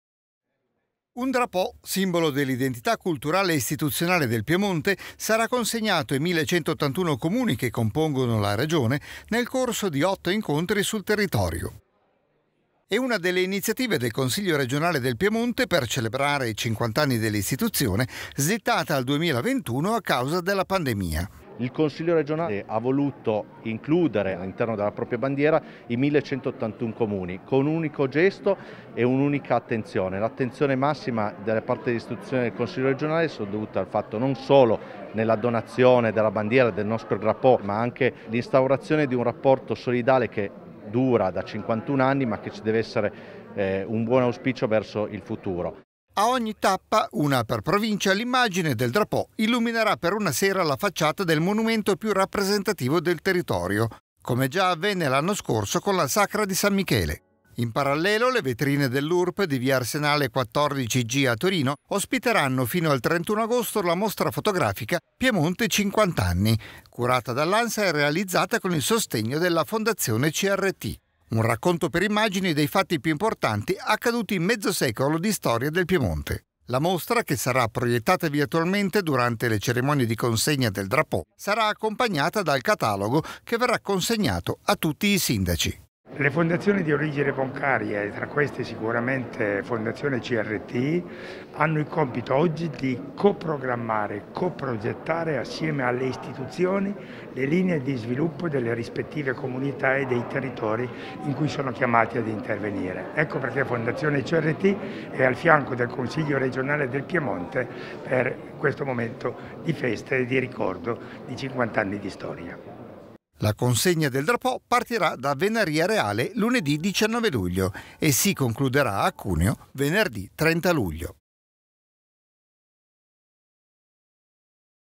Un drapò per l'identità del Piemonte
Il Consiglio regionale lo consegnerà ai 1.181 Comuni nel corso di otto incontri nelle varie province. Ad ogni tappa l'immagine del Drapò illuminerà per una sera la facciata del monumento più rappresentativo del territorio. Prevista anche la mostra fotografica "Piemonte 50 anni" nelle vetrine dell'Urp di via Arsenale 14/G a Torino.